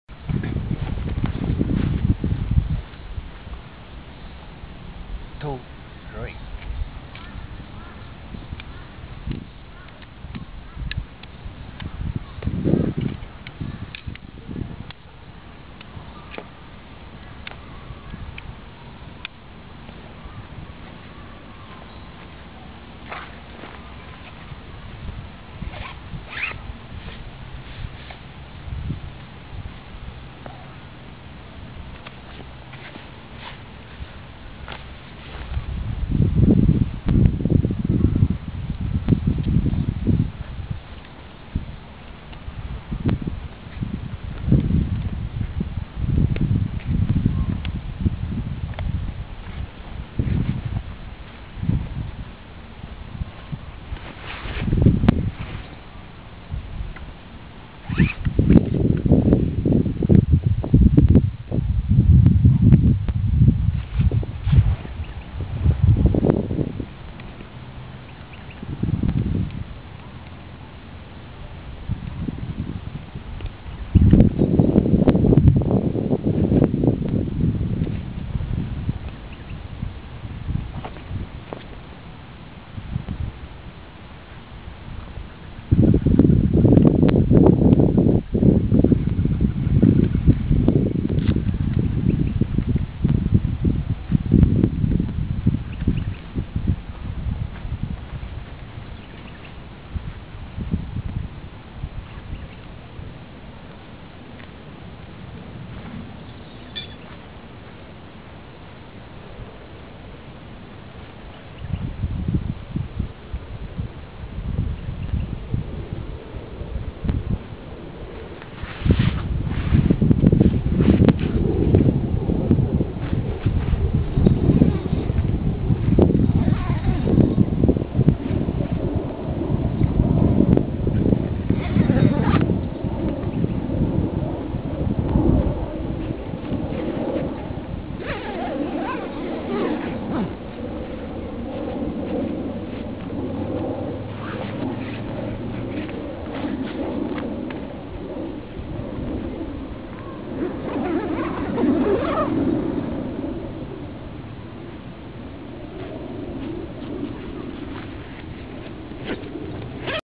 How to set up Canvas Swag, single or double good for camping hiking or motobike
The swag is specially designed for Aussie outback conditions, It's comfortable and durable which is perfect for car-camping and other occasions when you don't have to carry it!